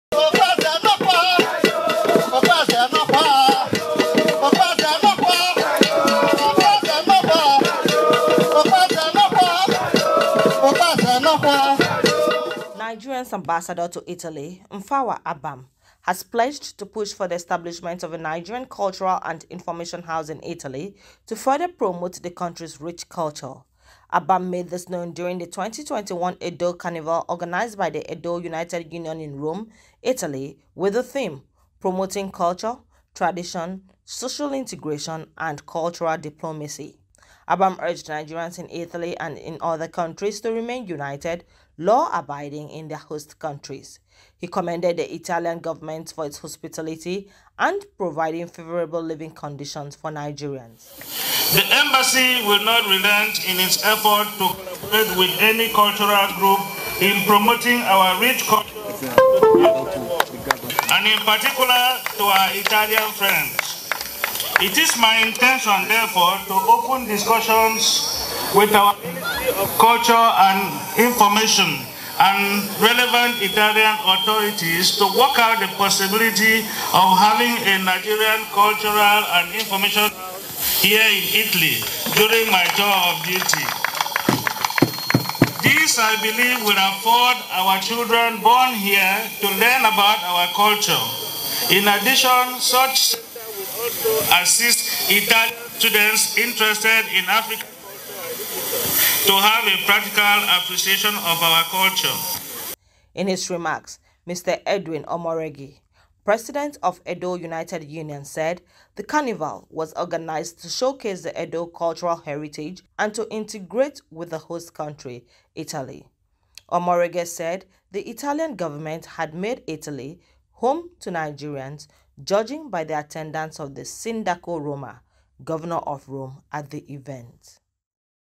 Envoy to push for establishment of Nigerian Cultural House in Italy
Nigeria's Ambassador to Italy, Mfawa Abam, pledges to push for the establishment of a Nigerian Cultural and Information house in Italy to further promote the country's rich culture.